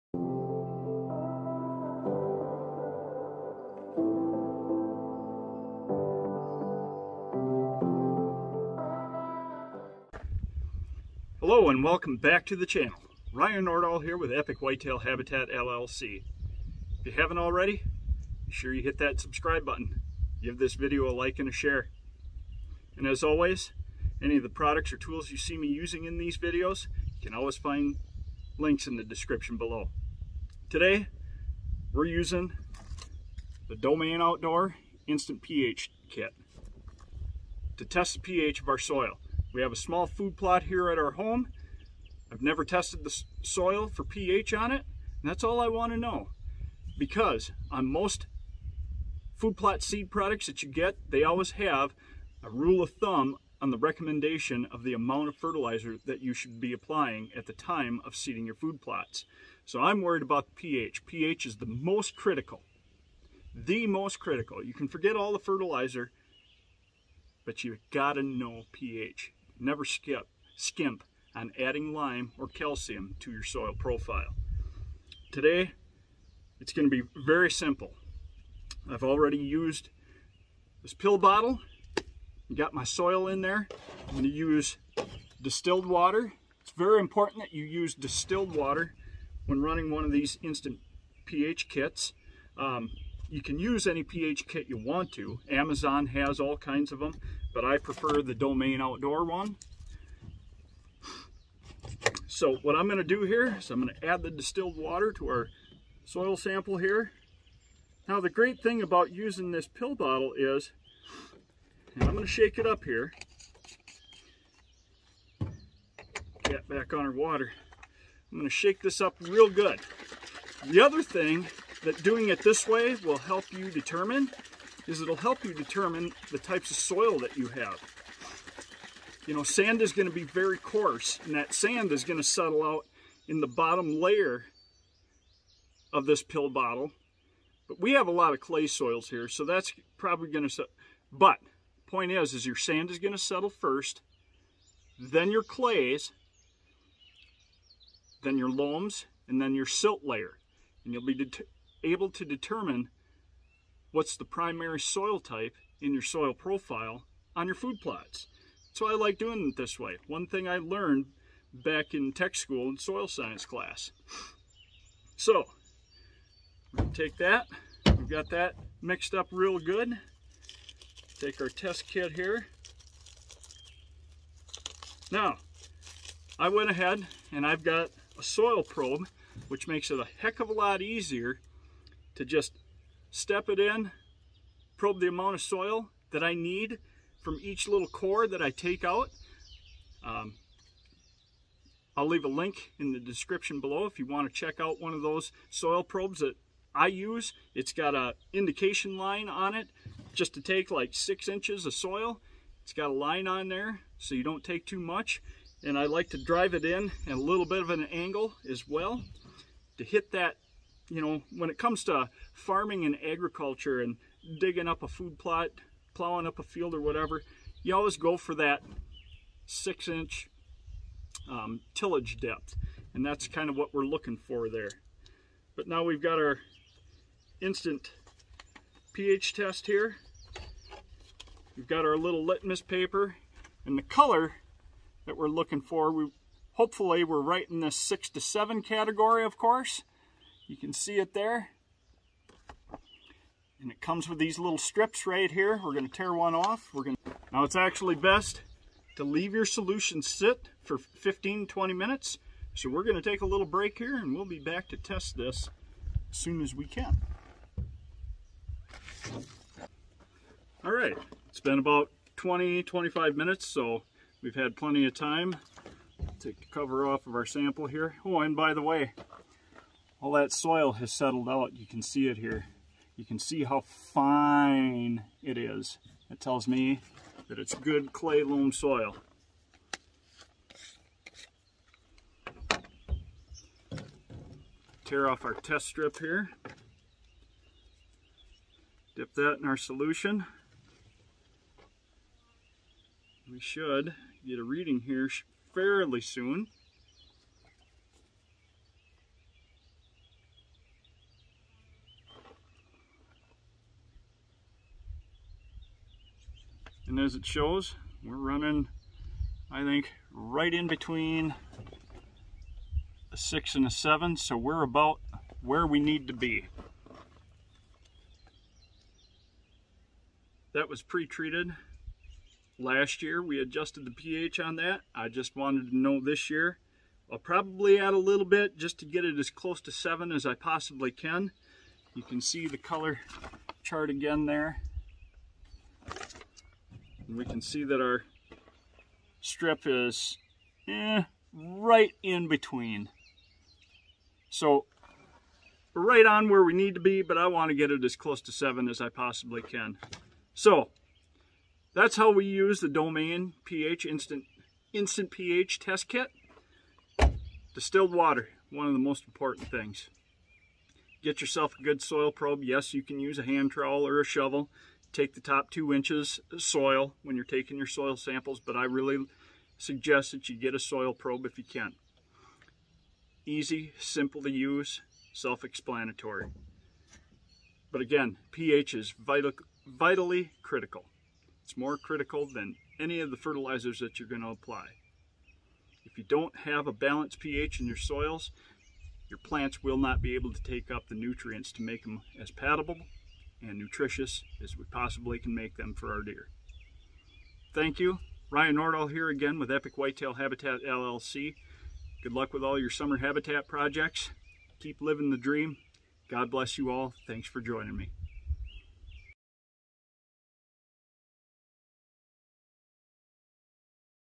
How to Use the Domain Outdoor Instant pH Test Kit
In this video, you will discover how to use the Domain Outdoor Instant pH Test Kit properly.

I even share an insight how to identify the correct soil types you are working with in your food plots.

36" Soil Probe

1 Gallon Distilled Water